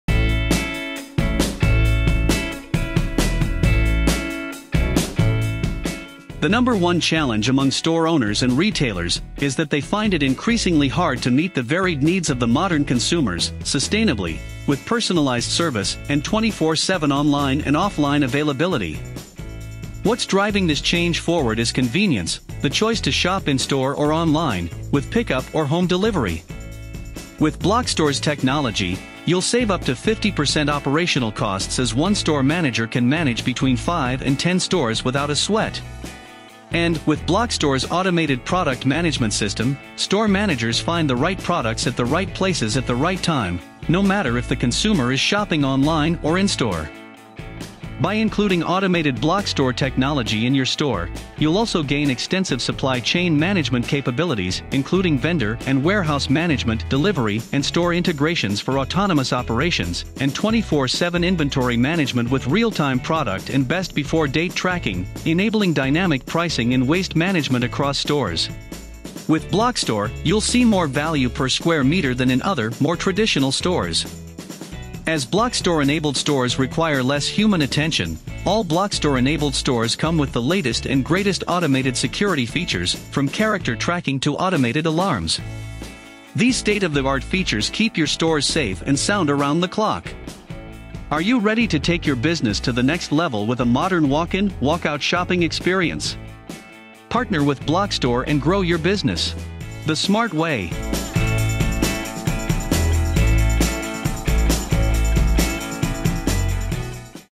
Blockstore Automated Grocery Stores | Full Solution
Blockstore solution – autonomous & automated grocery store retailing as a service.

A complete solution allowing grocery retailers to offer products when and where consumers want them - sustainably.

Covering multiple processes from warehouse to store - combining online opportunities and deliveries even to consumers.